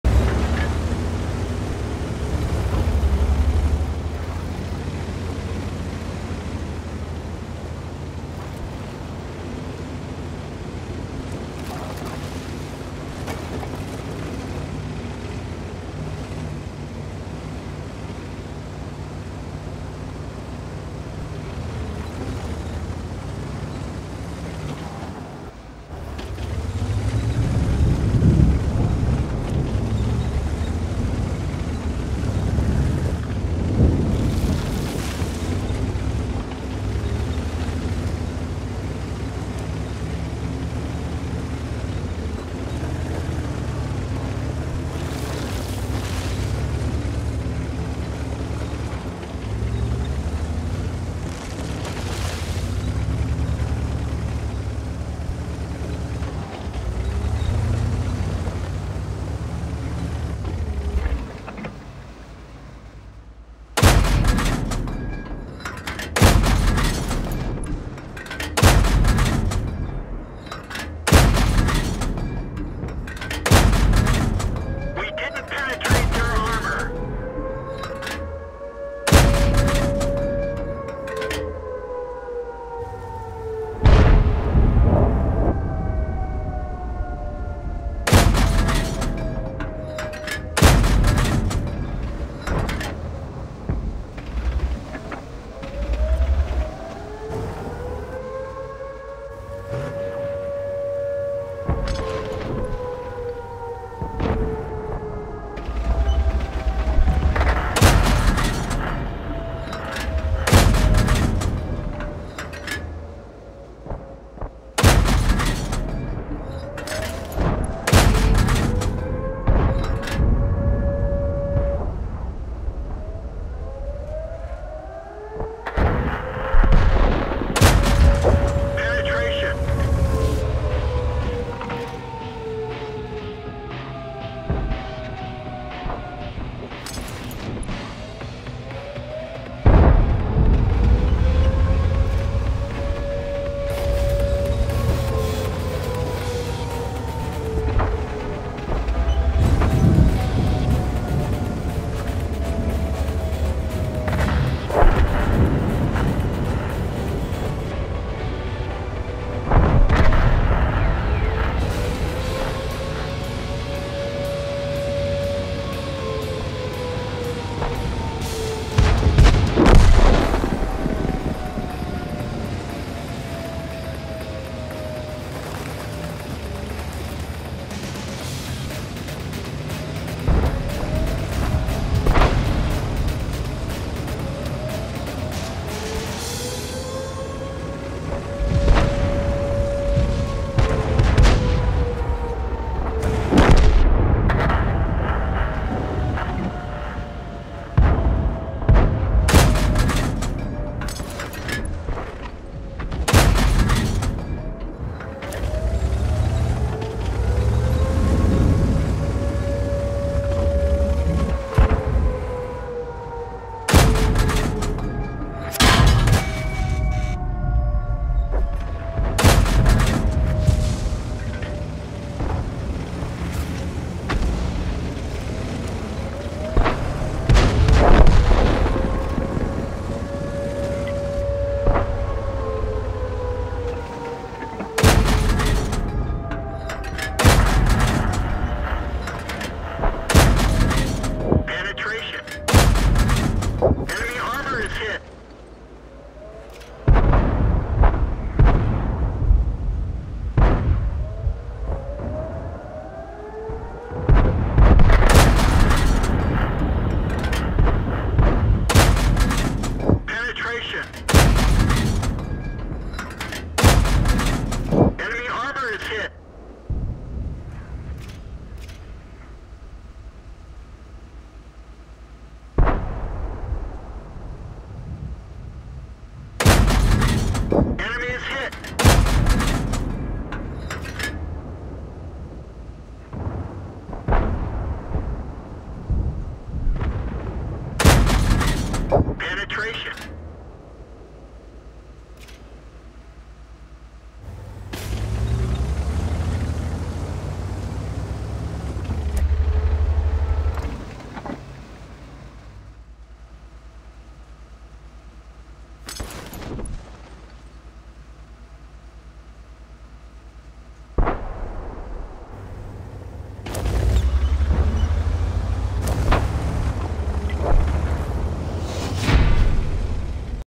T-34 Medium tank vs. Suppressive Fire with Wargaming Super Noob
Can you really put down some suppressive fire in a T-34? Well, just ask the IKV.